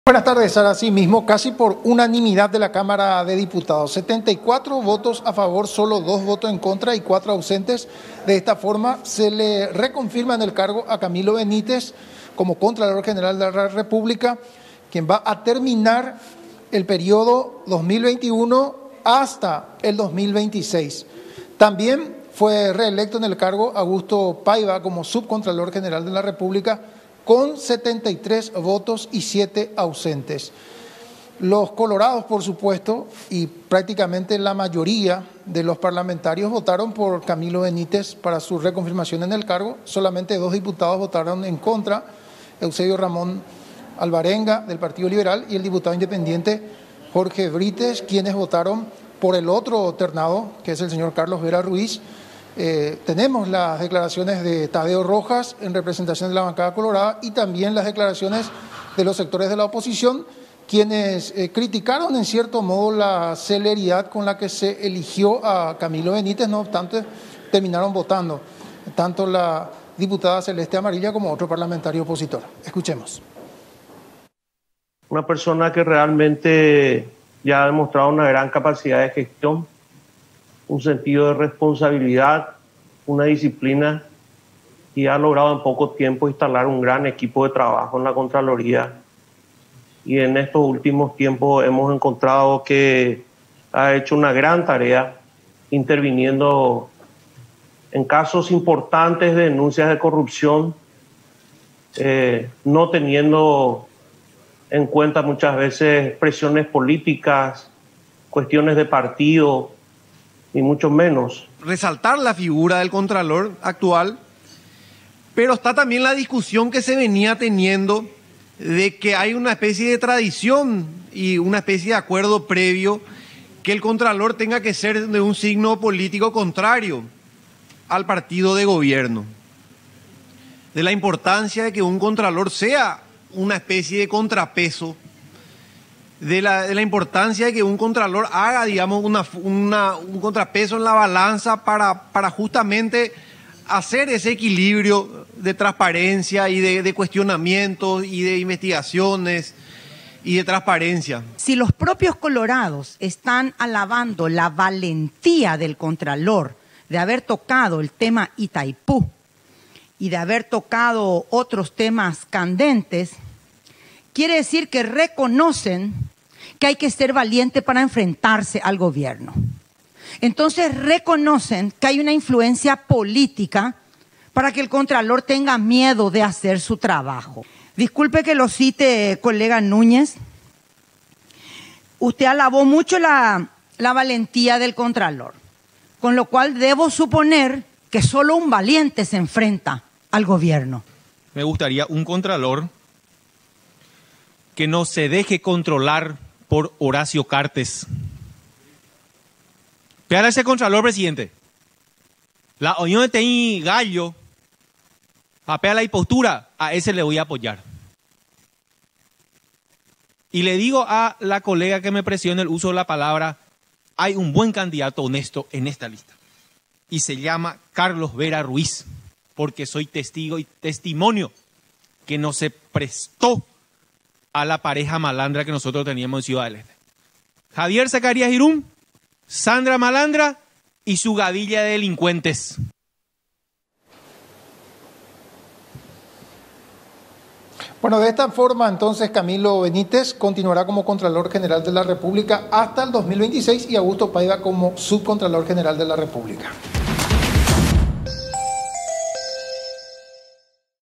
Diputados reeligen a Camilo Benítez como contralor
La Cámara de Diputados finalmente reeligió a Camilo Benítez (ANR) como contralor general de la República, con el apoyo de 74 de los 80 legisladores. Los legisladores eligieron a Augusto Paiva como subcontralor. Ambos prestarán juramento para asumir sus cargos para el nuevo período el próximo miércoles.